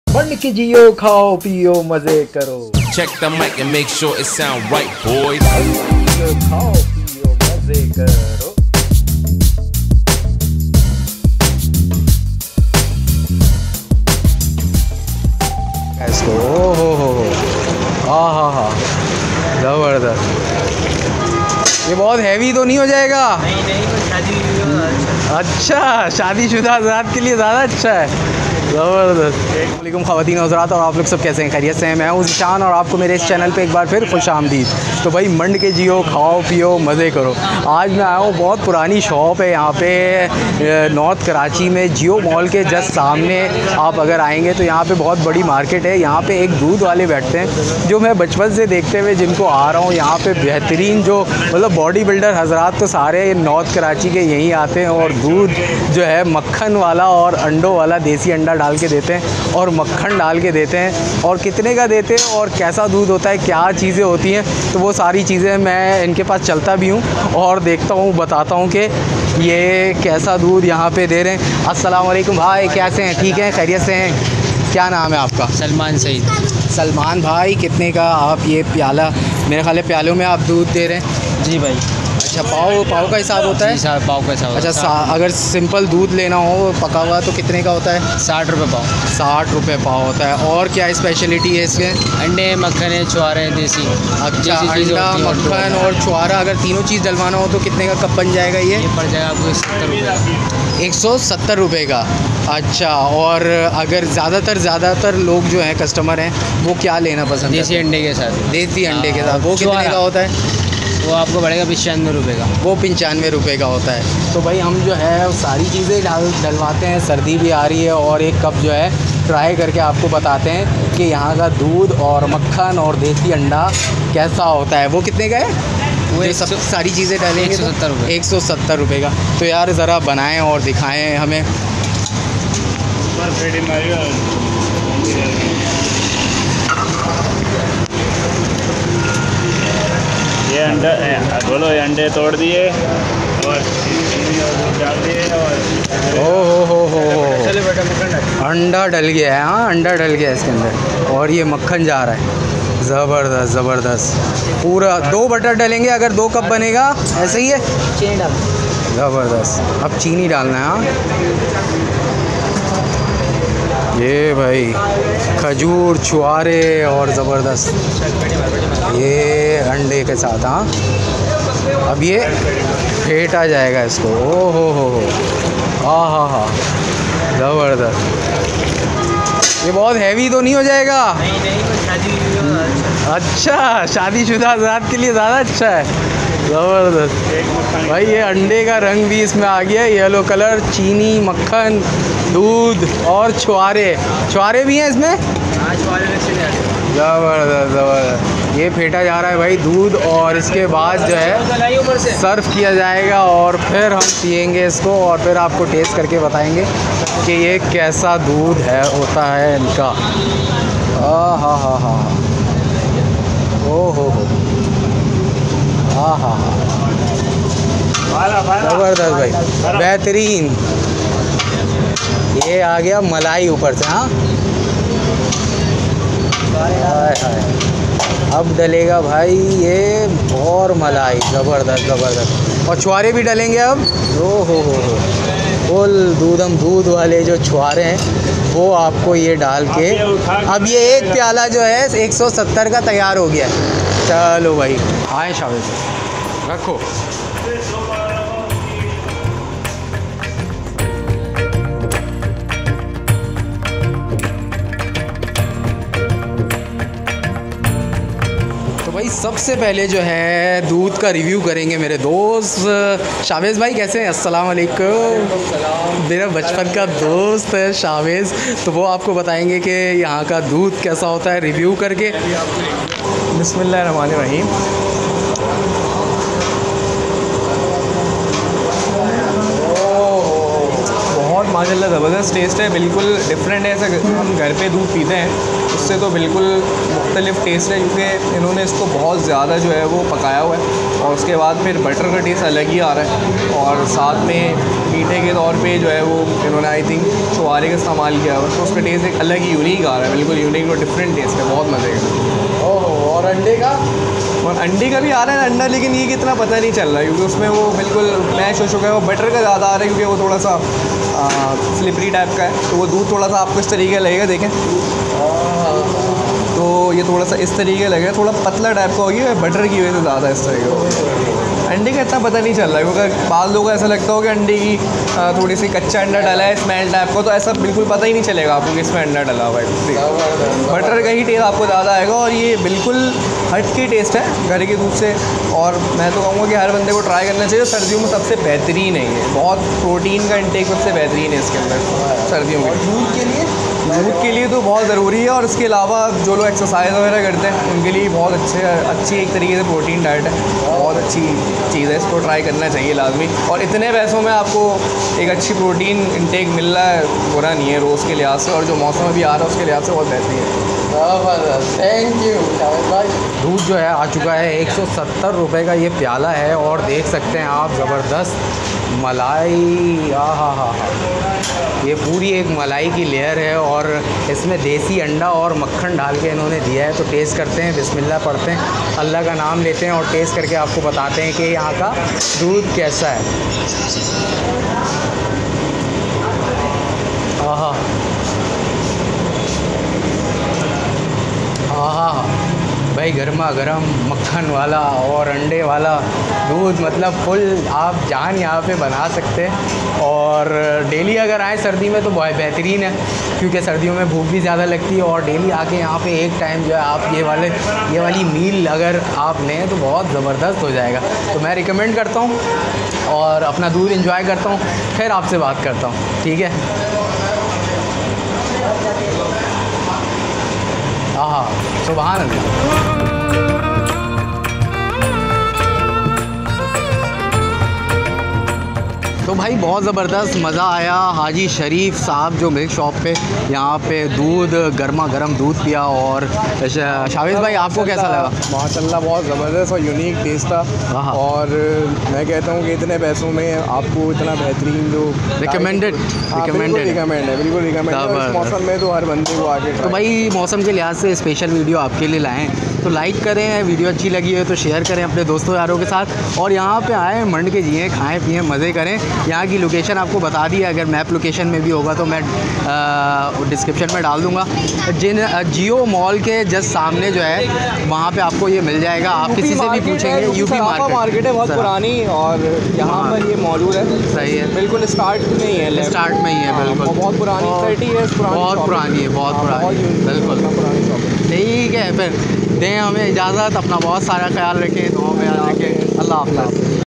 40 Years Old Shop Where They are Selling Fresh Milk with Butter, Desi Egg and Dates I Mand Ke Geo
Sardiyon ki Soghat I Khalis Desi Makhan, Khajoor aur Desi Anday Wala Doodh North Karachi Food Street Sarafa Bazar ki Mashoor Dukan "Haji Sharif Milk Shop"

40 Years Old Shop Where They are Selling Fresh Milk with Butter, Desi Egg and Dates.

Sardiyo me Apni Sehat aur Jaan Banany ka Behtareen Mouka ;)

Address: UP Sarafa Bazar, Opp Geo Mobile Market, North Karachi, Karachi, Sindh, Pakistan.

"Mand Ke Geo Khao Piyo Mazy Karo"

* Pakistani Vlogger
* Vlogger
* Karachi Vlog
* Food Vlogs
* In urdu
* In hindi